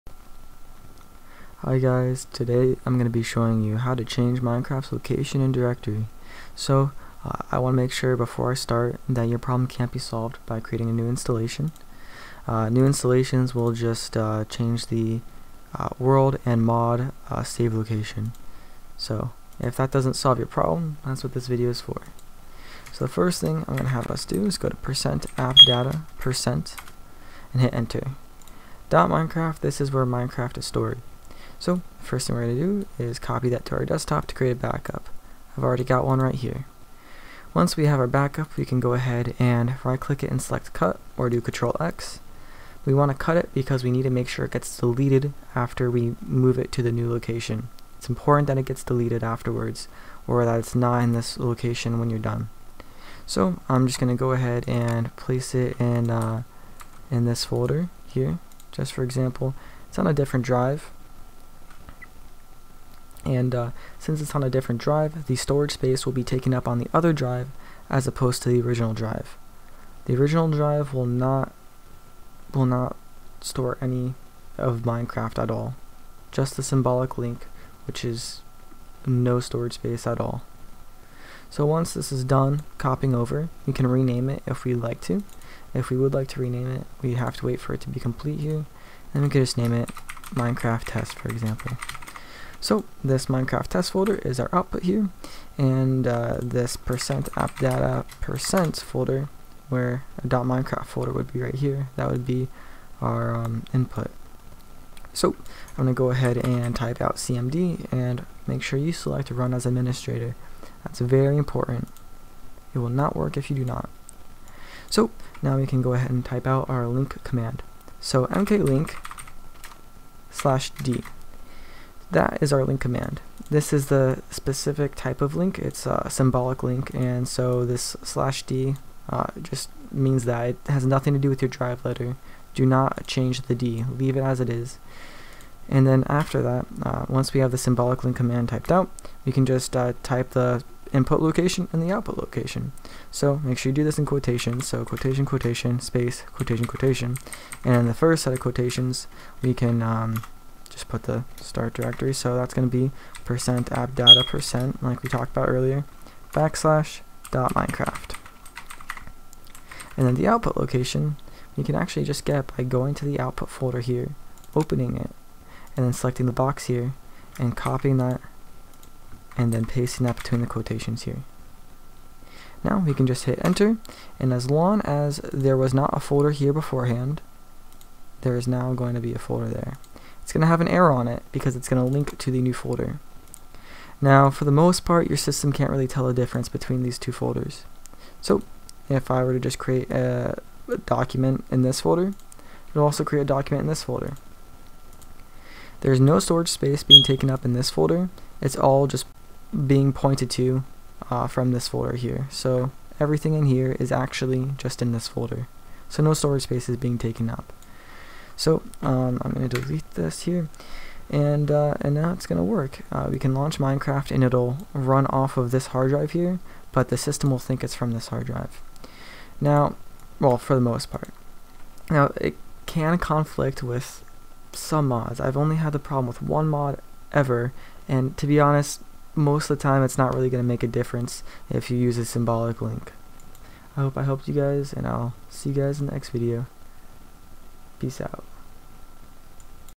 How to Change Minecraft's Location & Directory (2026)
mklink /D "%appdata%\.minecraft" "OutputLocation"

Consider leaving a like if I helped you out!